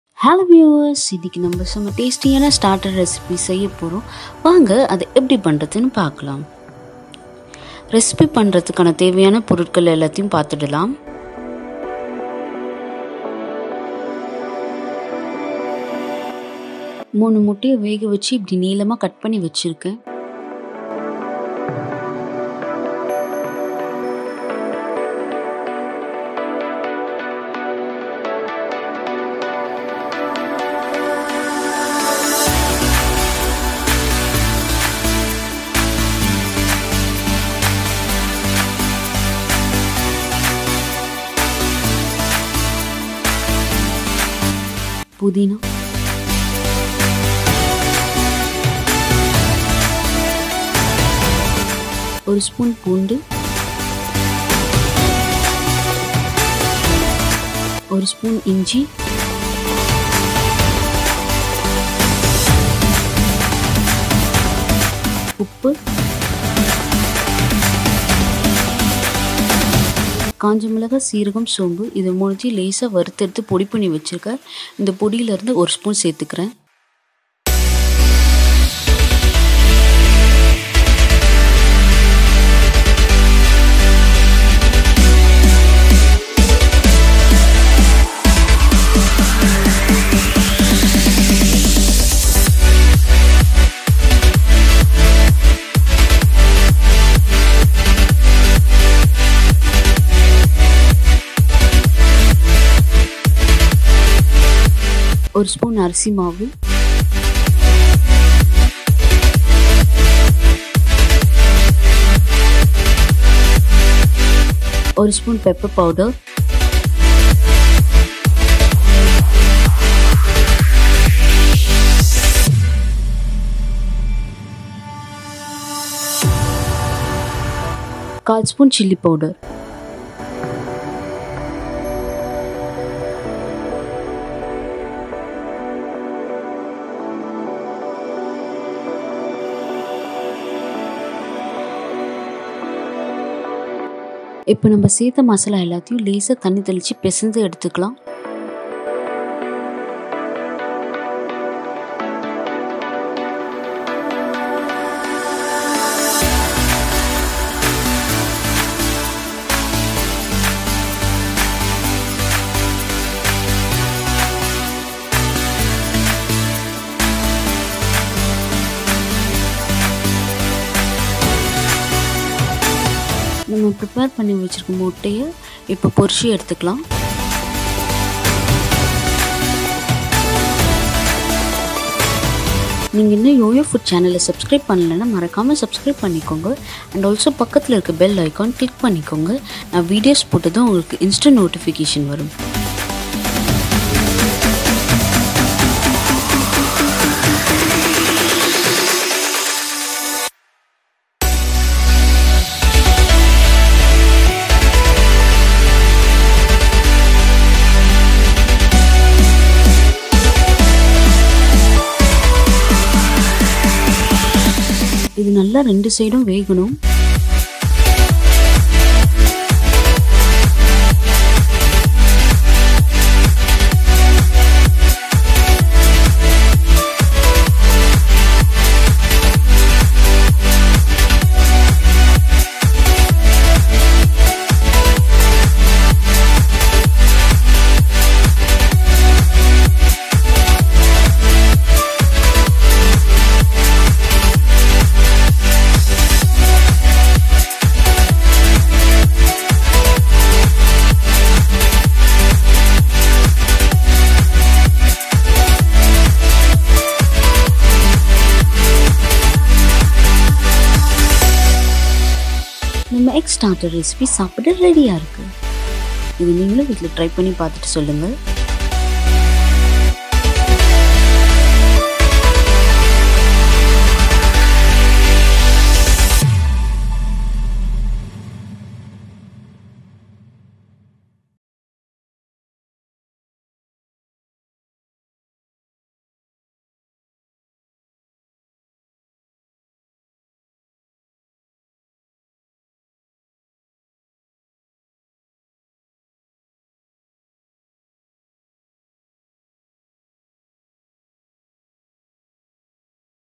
Fried Egg Starter Recipe | How to make starter with egg | Unique Egg Recipe
In this video we will see how to make egg fry starter recipe in Tamil. This is an easy starter recipe, made with the available ingredients at home.

Children will love this egg recipe, this can also served as snacks. Instead of the boring egg masala or omlette, if you try this fried egg starter/snack recipe everybody will be impressed.

Friends please do try this recipe at home and share this with your friends and family. Also please do share your feedback about the recipe in the comments below. All the best and happy cooking.

Eat, Sleep, Create - Yo Yo Food.